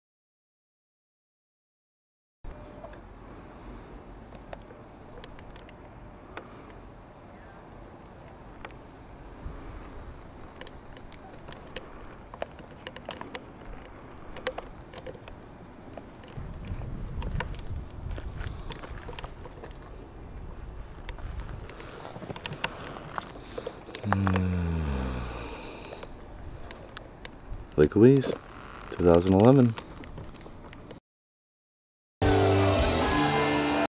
Lake Louise Gondola at mid-walkway in HD in Banff National Park Alberta, Canada!
Lake Louise Gondola mid-walkway HD Banff National Park Alberta Canada 2011 panorama panoramic mountain mountains Canadian Rockies